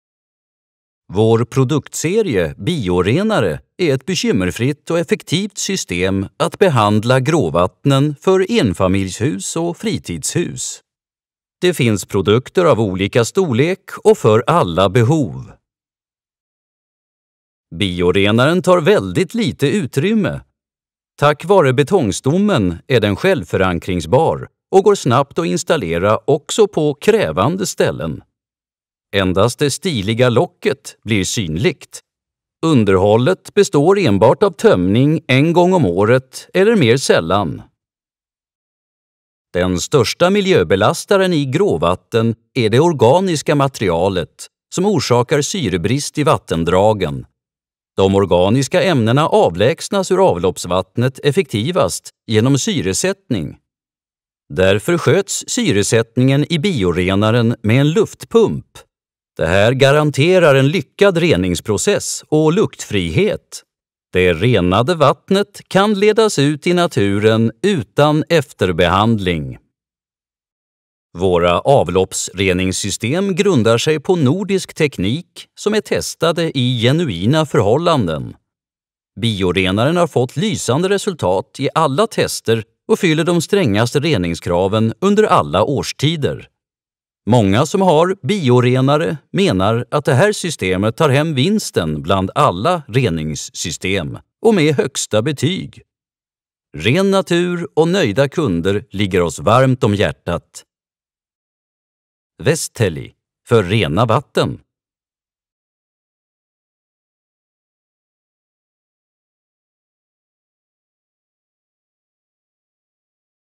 Klar etta – Vestellis Biorenare
Vestelli AB är marknadsledare i produktutveckling och tillverkning av BDT-avloppsvattensystem i Finland. Vårt BDT-reningssystem, som grundar sig på nordisk teknik, är testat i äkta förhållanden.
I oberoende användarundersökningar vinner Vestellis Biorenare alla andra rengöringslösningar med högsta betyg.